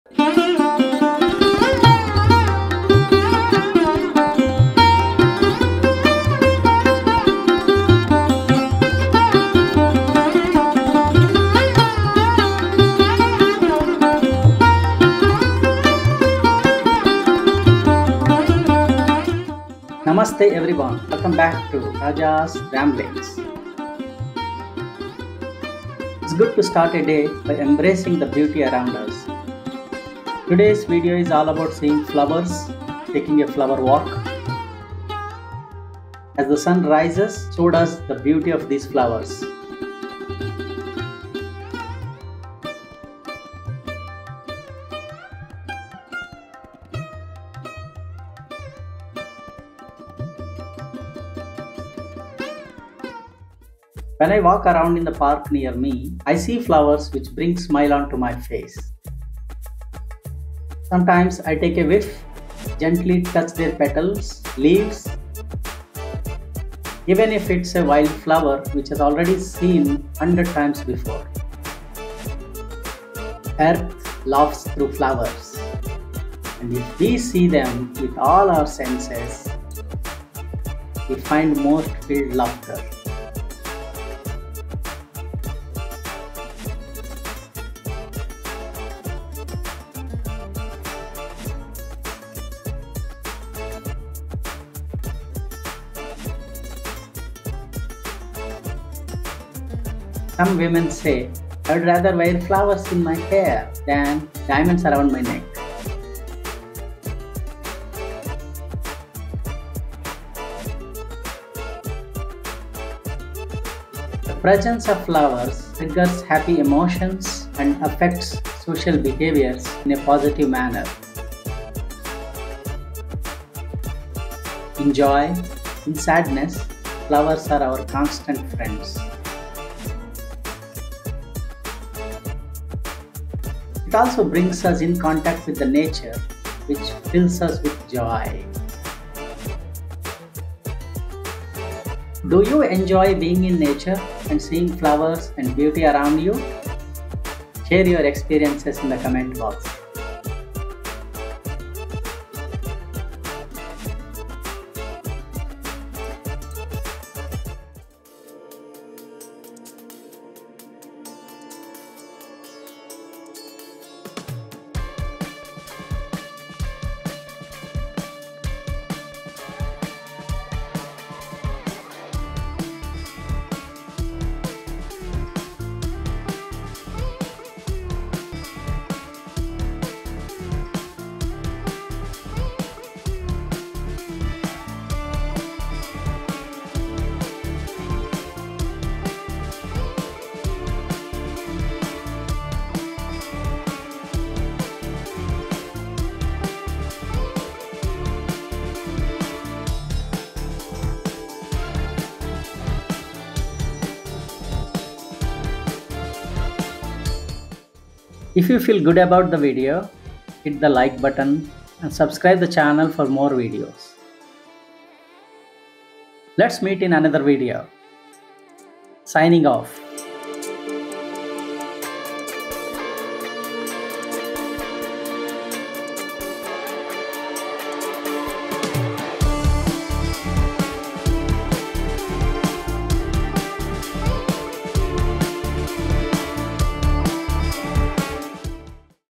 Flower Walk I Vlog I Embrace Nature I
Its good start a day by embracing the beauty around us.
This video is all about seeing flowers and taking a flower walk
When I walk around in the park near me, I see flowers which bring smile on to my face.
Earth laughs through flowers and if we see them with all our senses, we find most filled laughter.The presence of flowers triggers happy emotions and affects social behavior in a positive manner.In joy and in sadness, flowers are our constant friends.It also brings us in contact with nature which fills us with joy.

Do you enjoy being in nature and seeing flowers and beauty around you?